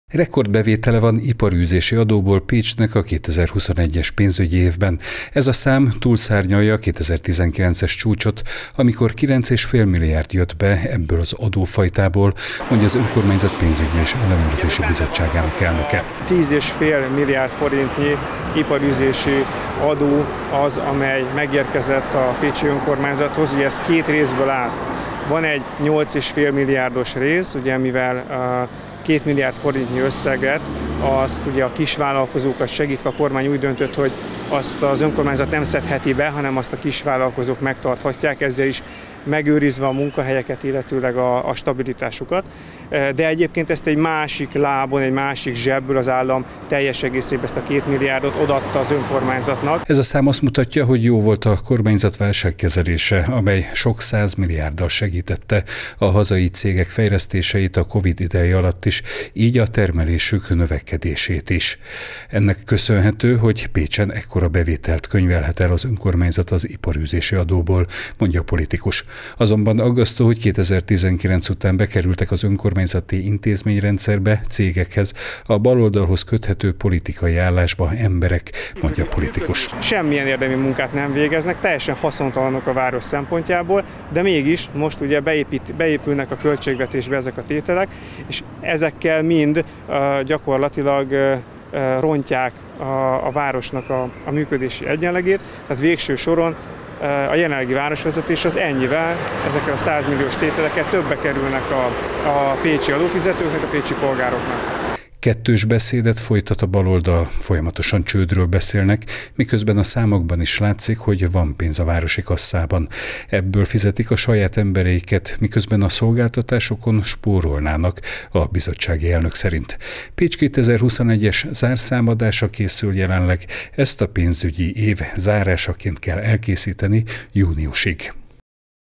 Adóbevételi csúcs Pécsen
Rekord összegű iparűzési adó, illetve maradvány összegek a 2021-es évről - ezeket tartalmazza az önkormányzaton készülő zárszámadás. Ebben a 2021-es pénzügyi évet zárják le tételesen. A dokumentum a napokban került a Pénzügyi Bizottság elé, ahol kiderült: 10,5 milliárd forint folyt be az iparűzési adóból, ami rekordnak számít. Mint ismert, a baloldalon folyamatosan a város csődjéről beszélnek. A bizottsági elnök szerint kettős beszédet folytat a baloldal: a szolgáltatásokon spórolna a pénzhiányra hivatkozva, miközben a politikai állásokba tett embereinek százmilliókat fizetnek ki.